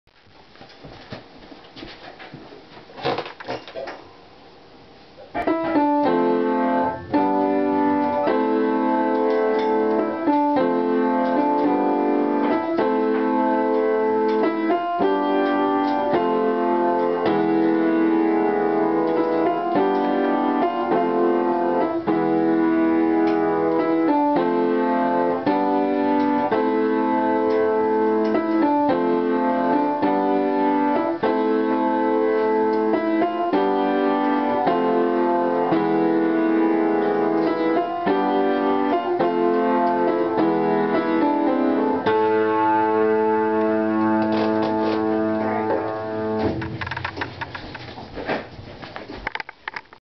Me playing the piano
Me(jenny ) playing a song I made up on the piano I made another video of this song but it isn't as good as this one so enjoy.